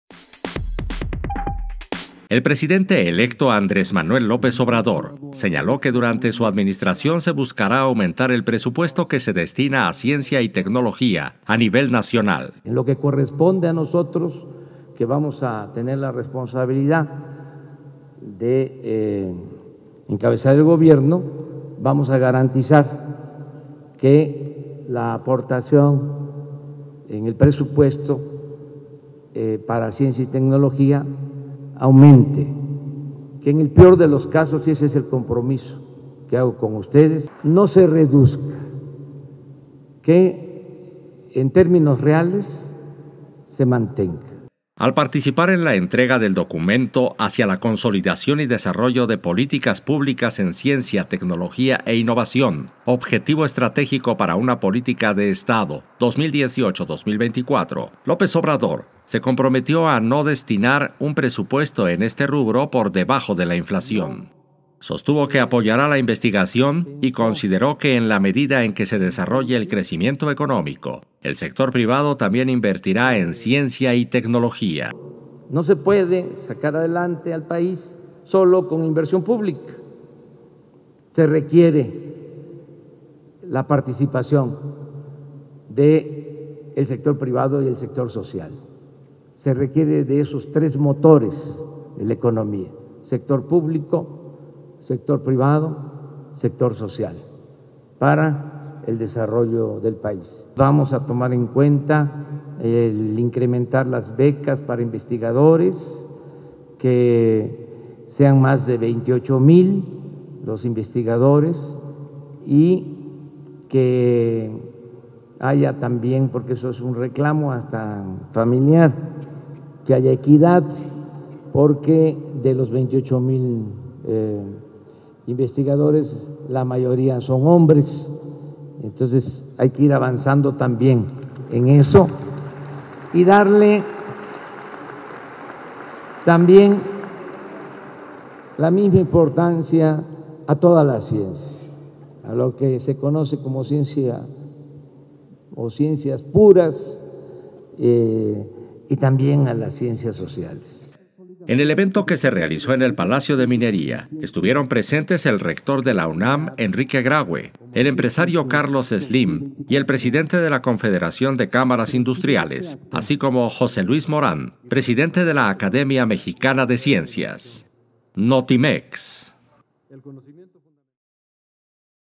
Ofrece López Obrador buscar más presupuesto para ciencia y tecnología
La comunidad científica entregó al presidente electo el documento con propuestas para el impulso de la ciencia, tecnología e innovación.